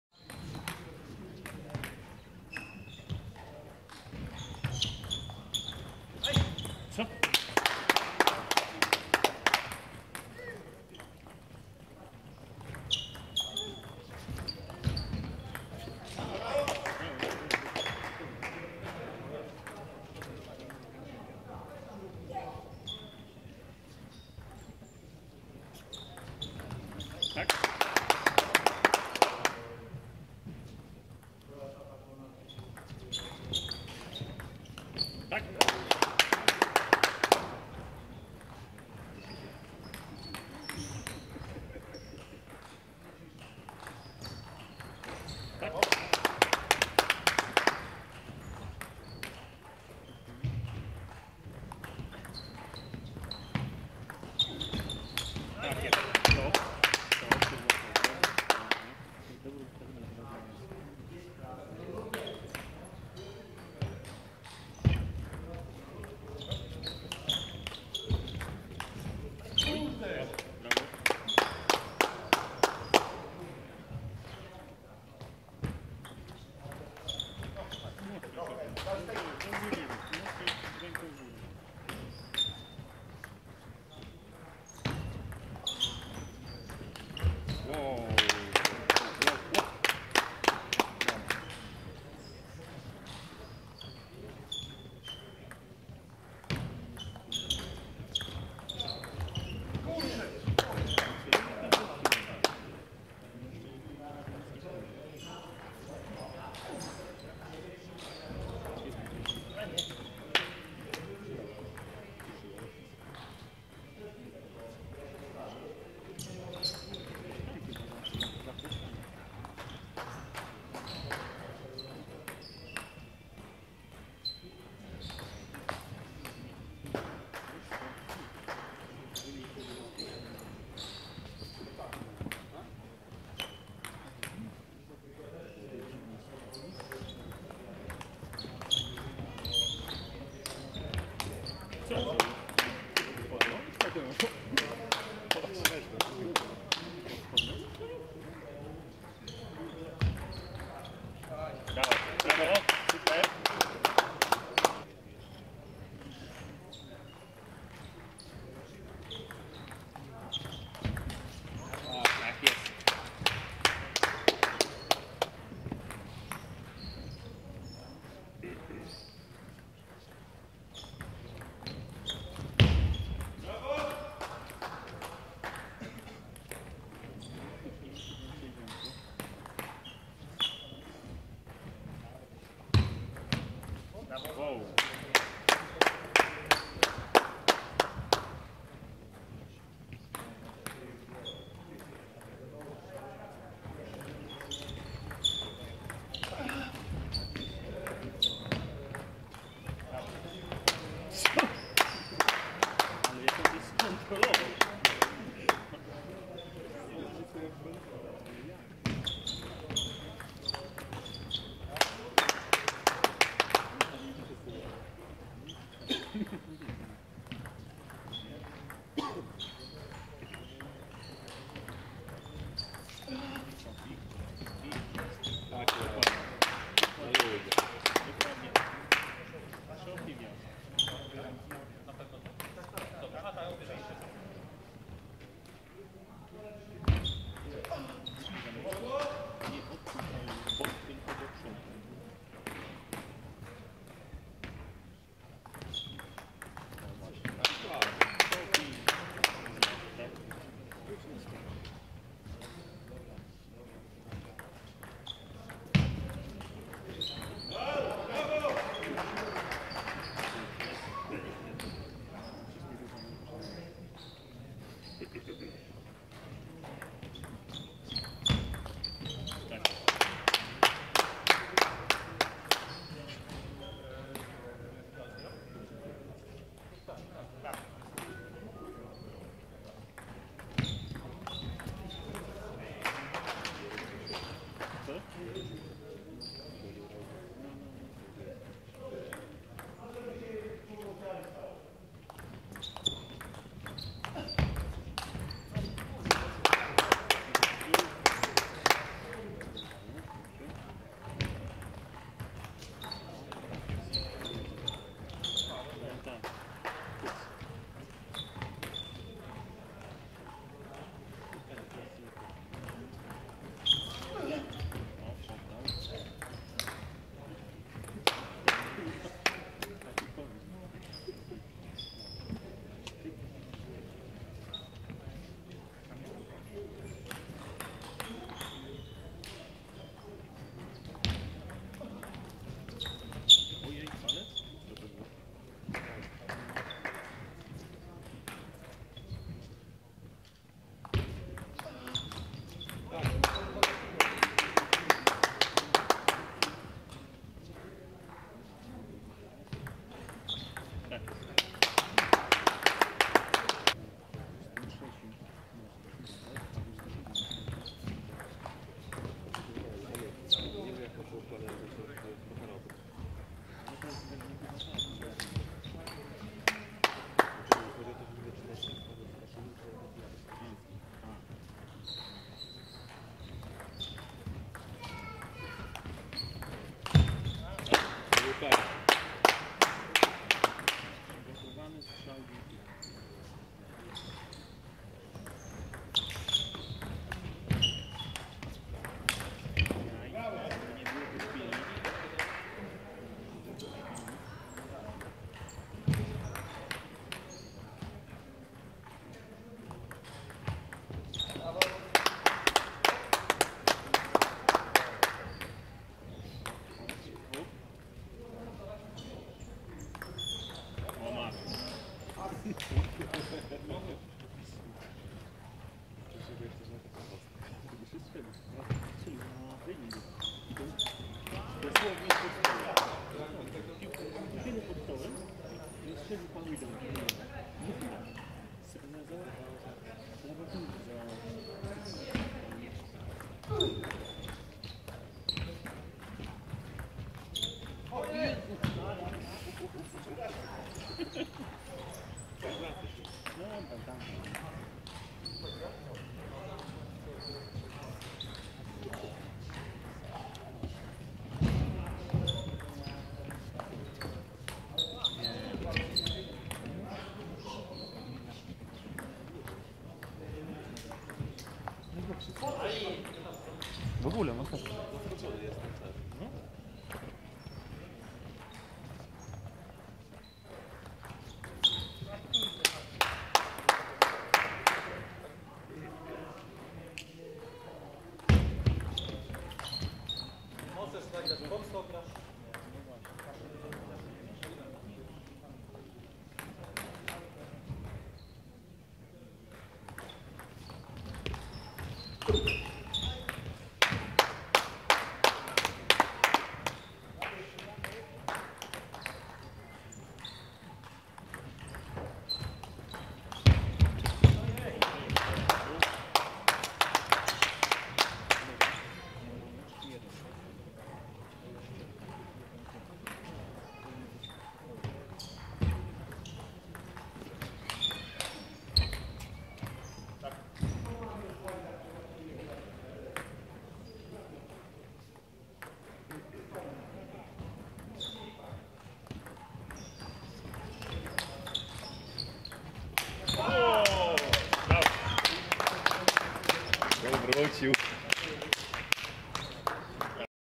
Grzegorz Kaszuba, Tomasz Zabielski - Paweł Gałecki, Michał Markowski (Puchar Polski 2020/2021)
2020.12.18 ULKS Macovia Maków Mazowiecki - UKS Budowlani Góra Kalwaria  G.Kaszuba, T.Zabielski - P.Gałecki, M.Markowski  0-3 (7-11, 6-11, 5-11)